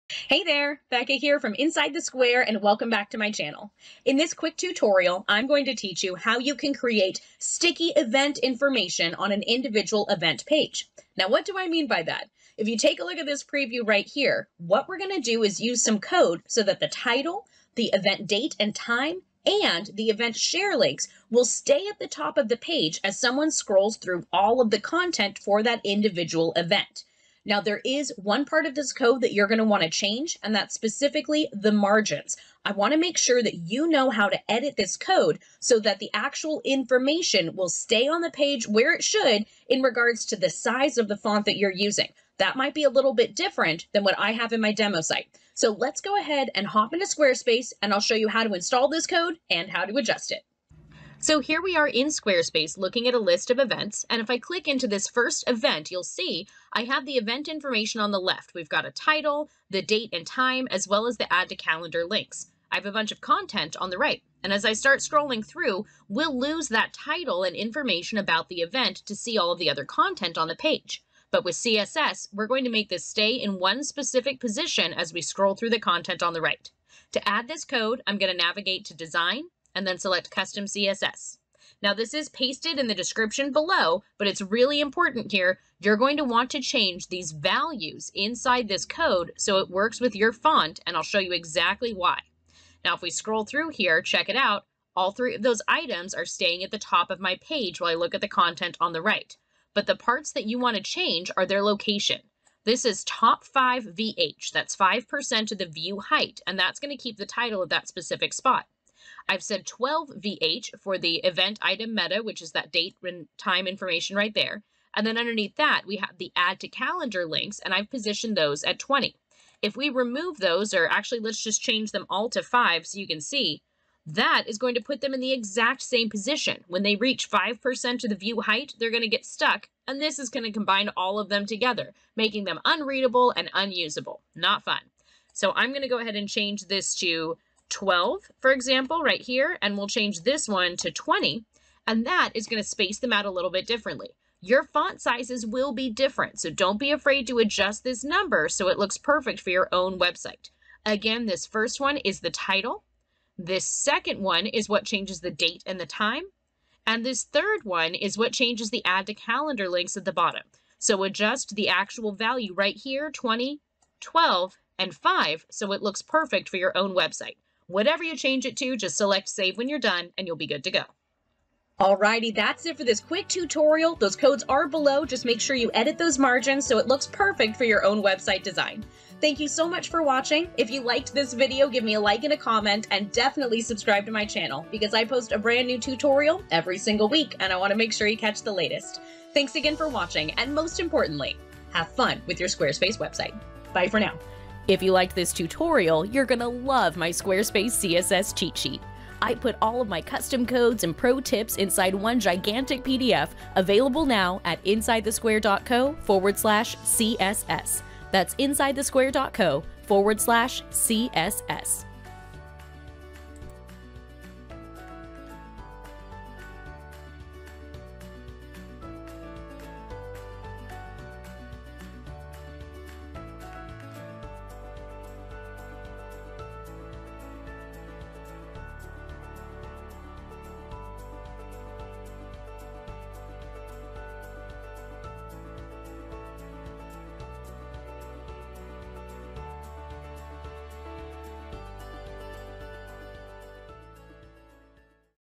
How to keep event info visible on a scroll in Squarespace // Squarespace Sticky Event Info Tutorial
This quick tutorial will show you how to create “sticky” event info so that it stays at the top of the screen on an individual event page.
This is just the start of all the cool things you can do with custom CSS! Learn the basics in my free class
There is a part of this code you’ll need to change to make sure it looks perfect on your own site. Your values will need to be larger or smaller depending on the font that you are using. I recommend using an absolute value like PX or REM.

Here is the code featured in this Squarespace tutorial:

.eventitem-title {position:sticky!important; top:5vh!important}
.eventitem-meta{position:sticky!important; top:15vh!important}
.event-meta-addtocalendar-container.eventitem-meta{position:sticky!important; top:25vh!important}

🤩 Download over 60+ pages of custom codes and pro tips for customizing Squarespace in one awesome PDF. Order the Squarespace CSS Cheat Sheet, available now